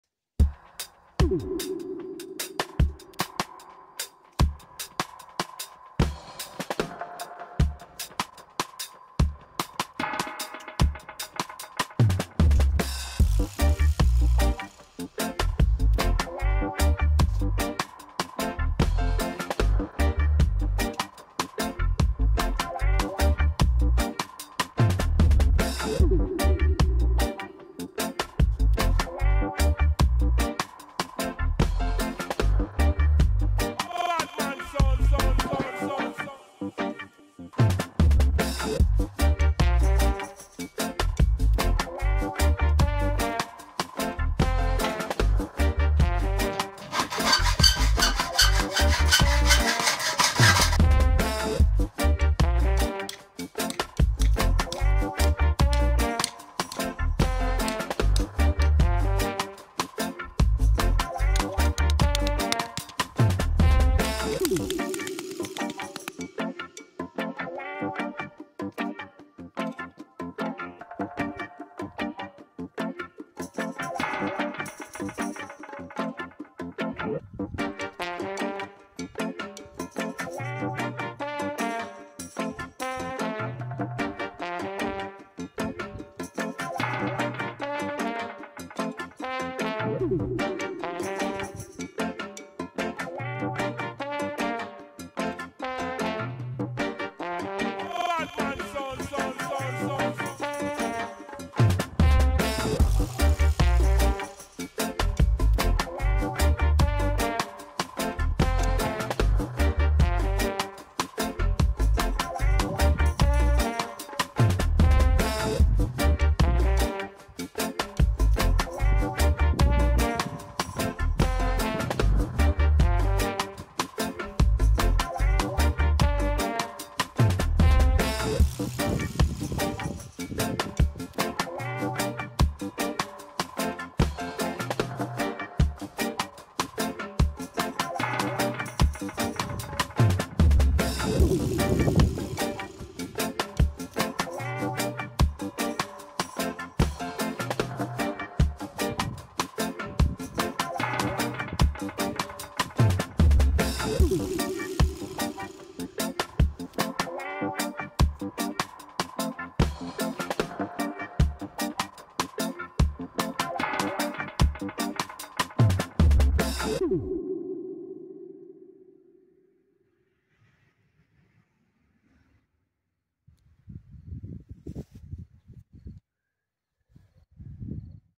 Ce putem face din nuiele de alun?
In vechime din nuiele de alun se impleteau cosuri sau garduri pentru imprejmuiri.Este un material ecologic care se gaseste din abundenta in padurile noastre.